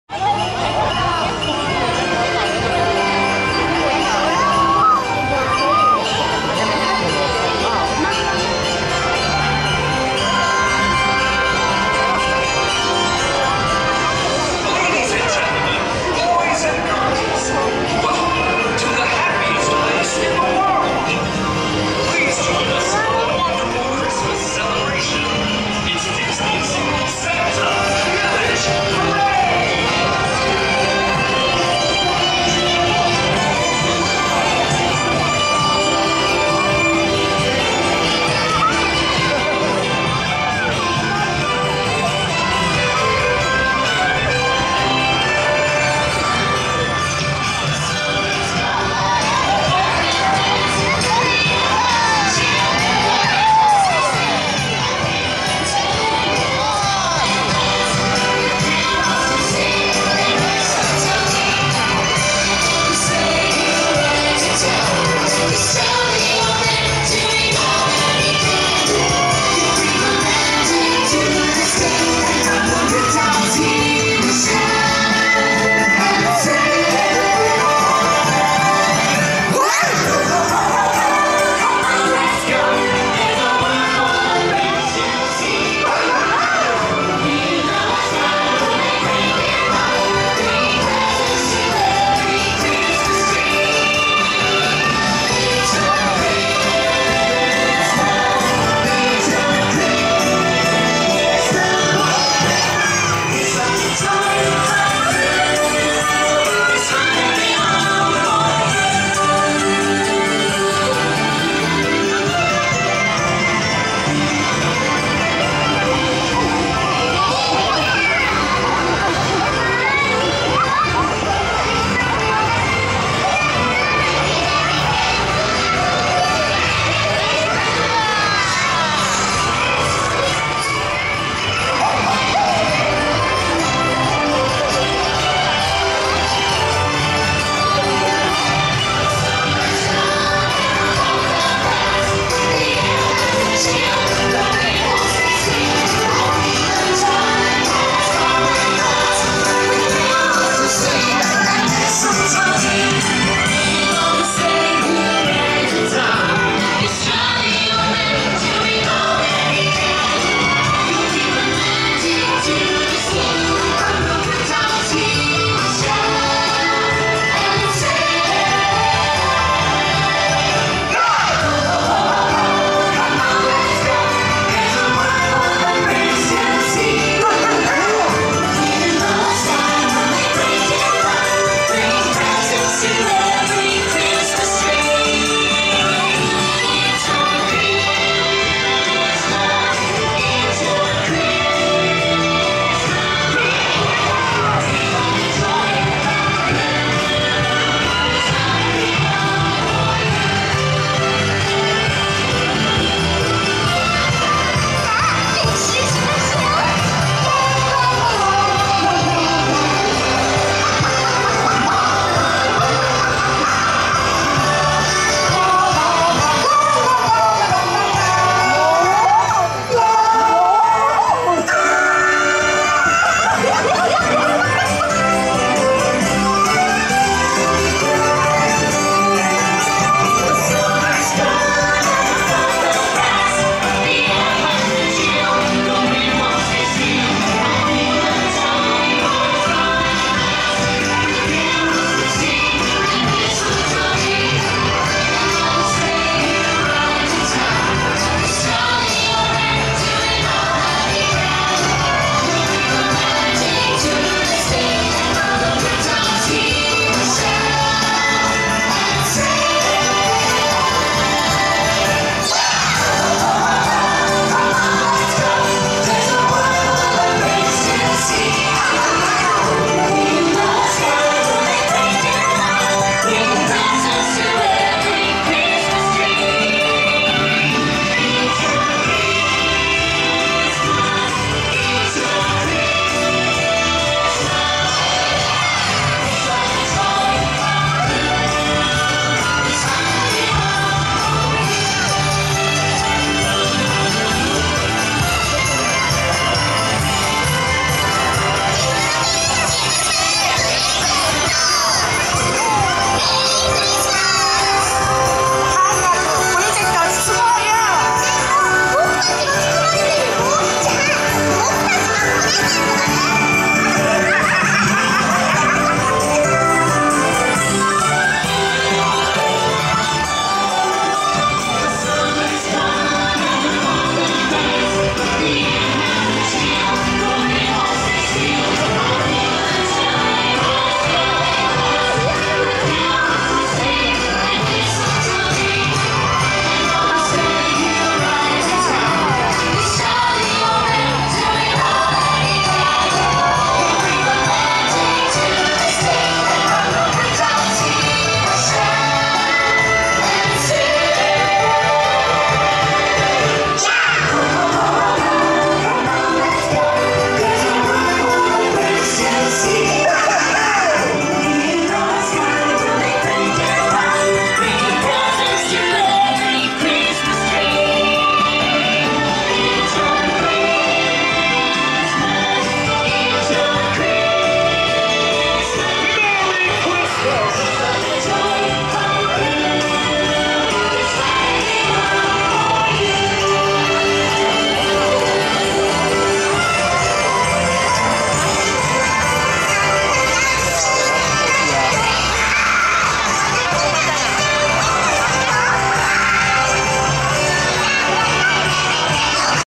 ディズニー・サンタヴィレッジ・パレード2013 (トゥーンタウン方面へ通過の様子)
2013年11月27日午後の部(15時40分スタート)ウエスタンランド側で撮影後急いで移動、ここで通過の様子を撮ってみました(16時05分)。